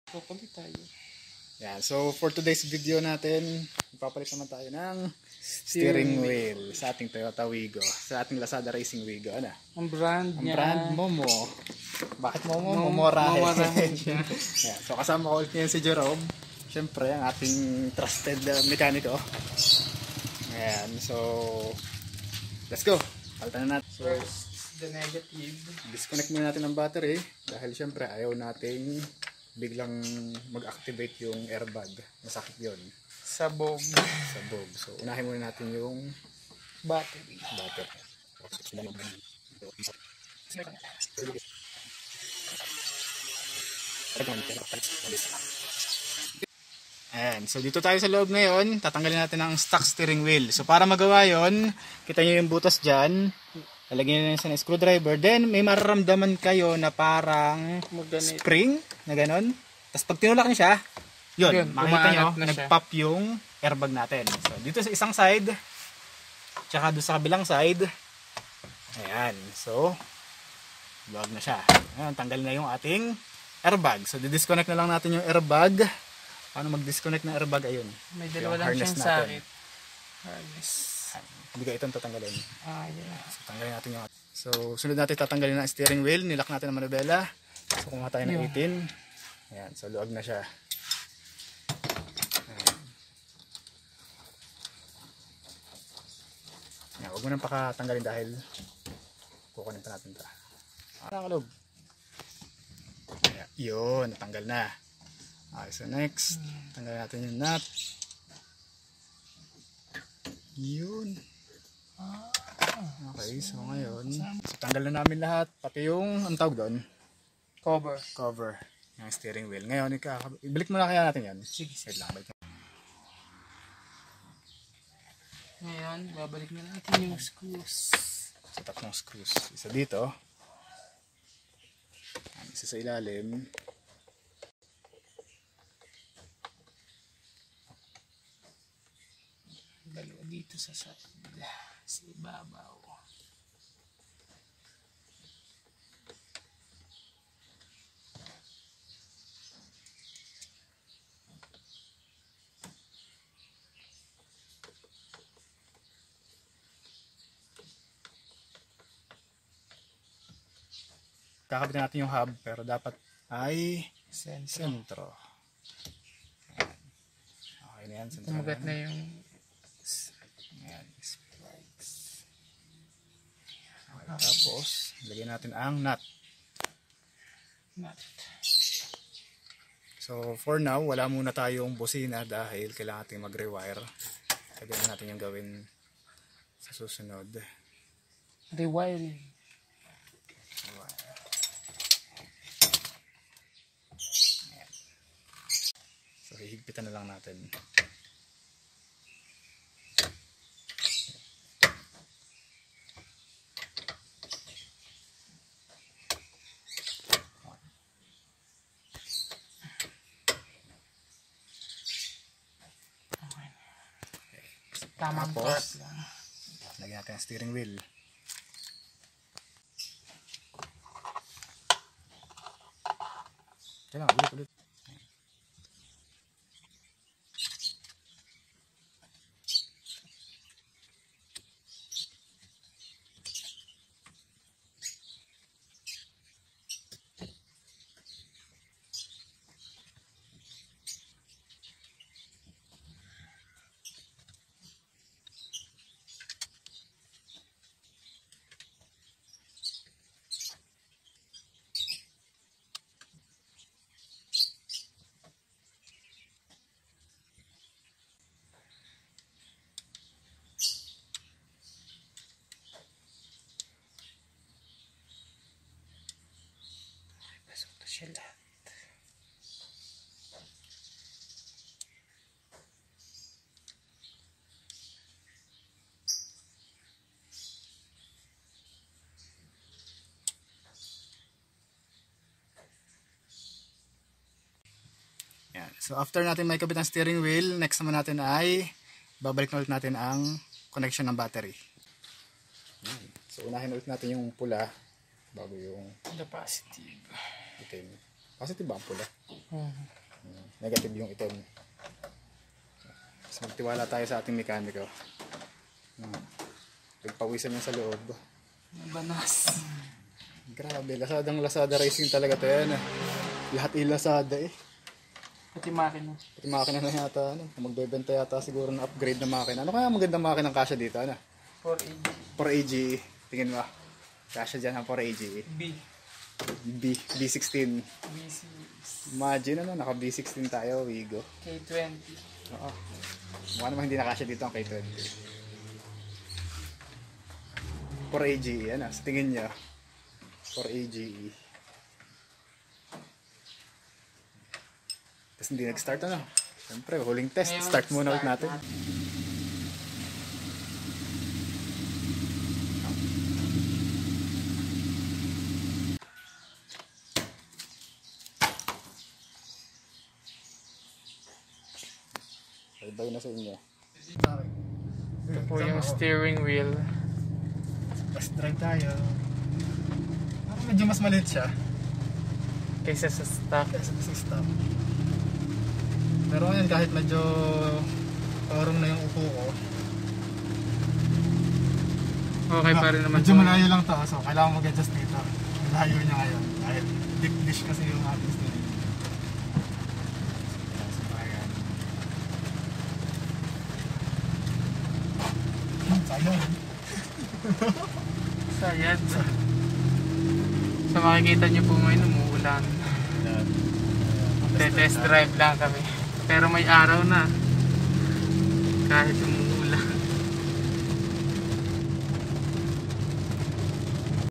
changing stock toyota wigo into momo(rahin) steering wheel
from stock toyota wigo to a momo steering wheel